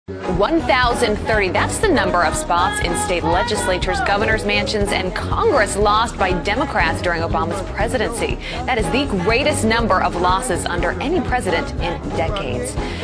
Obama's Legacy IN 13 SECONDS
Barack Obama Success - Democrats Lost 1,030 Seats During Presidency; Russians Did That Too? Video By: Mark F @Immigrant4trump